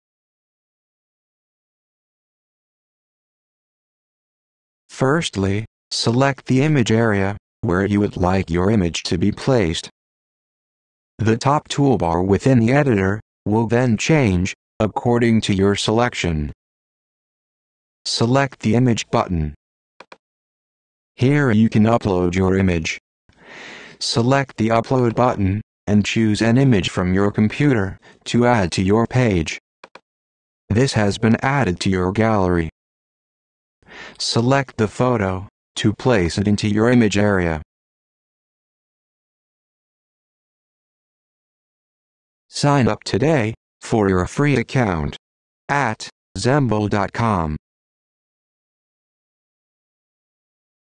How to add an image
Video guide on how to add images to your project in Zembl online authoring tool : How to add an image.
for a free account!